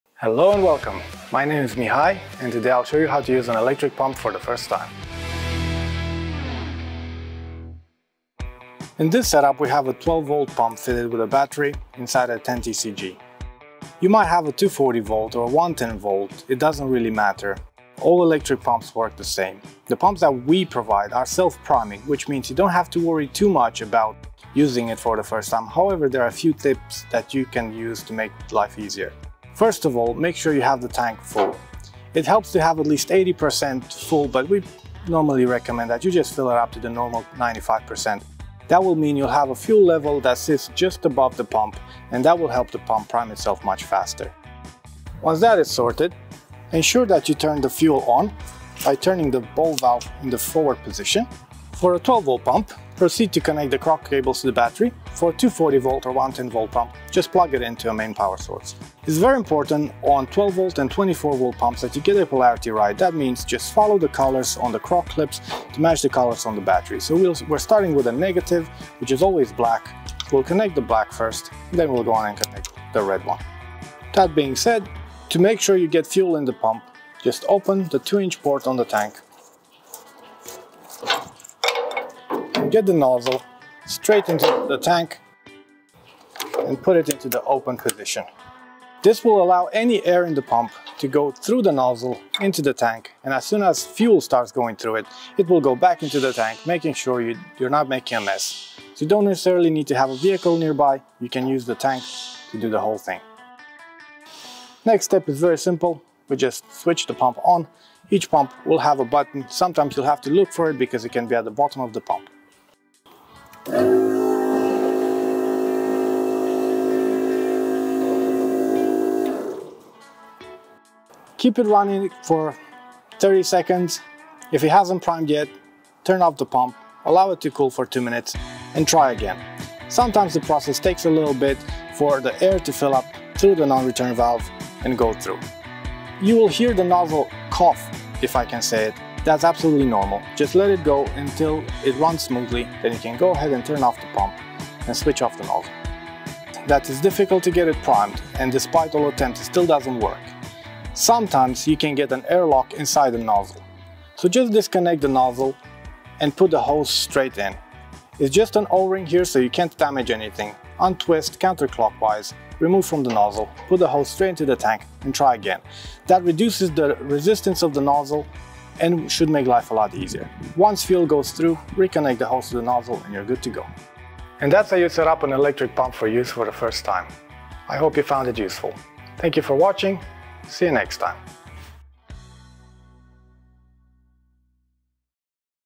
How To: Set Up An Electric Pump | UK
Hints and tips of how to set up an electric pump. Get the most from your equipment.